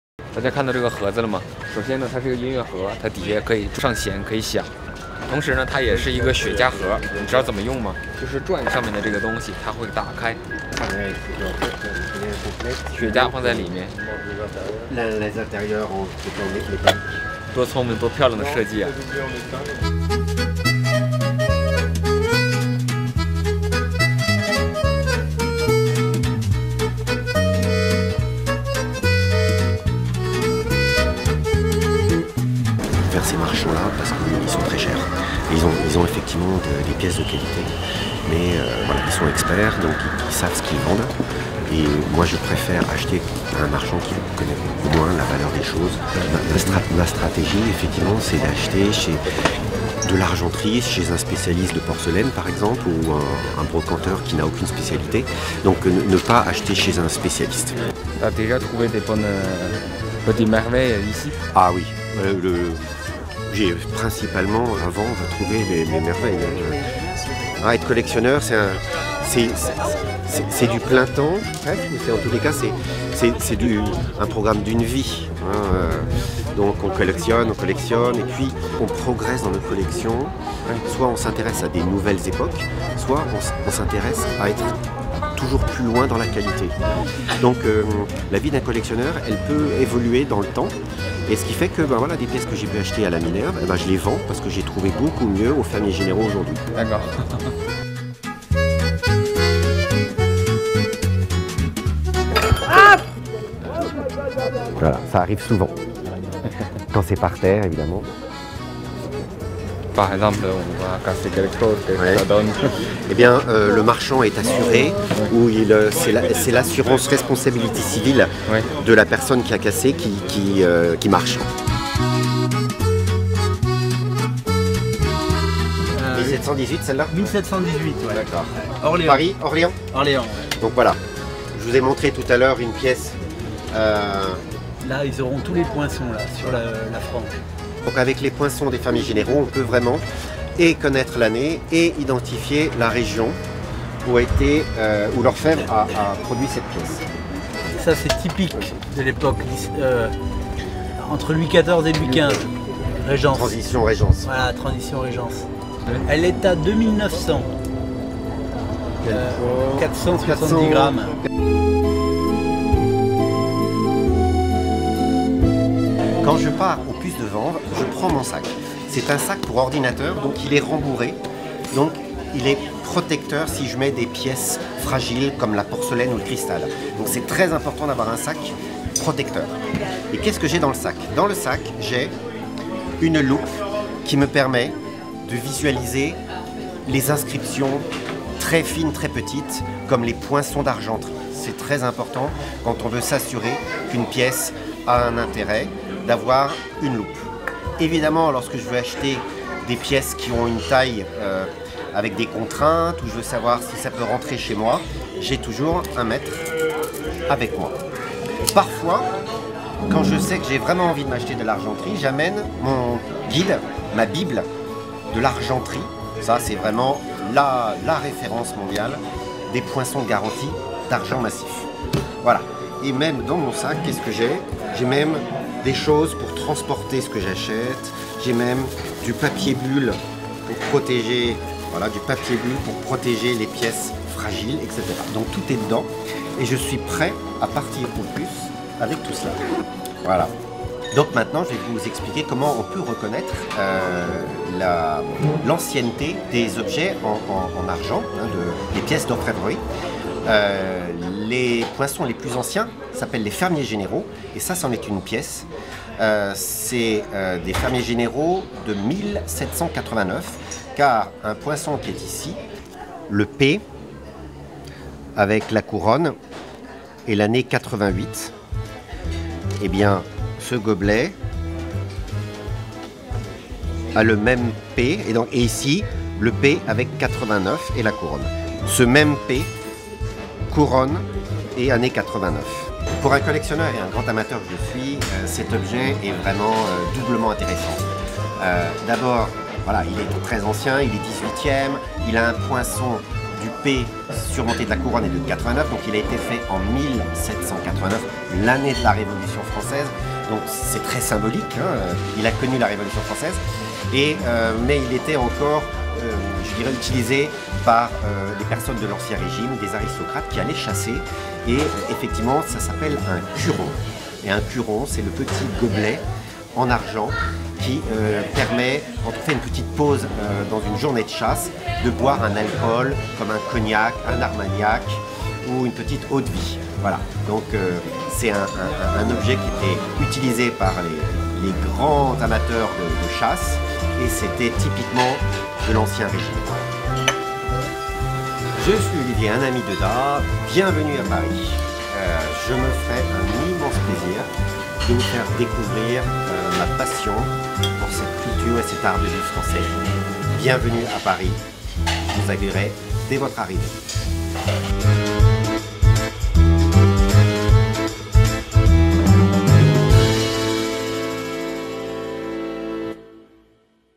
COMMENT RECONNAÎTRE LA VALEUR DE L'ARGENT [VANVES]
Seriez-vous deviner le prix des objets dans cette vidéo?

Venez découvrir la suite de notre excursion au marché aux puces de Vanves avec Meng Da et Olivier Rérolle !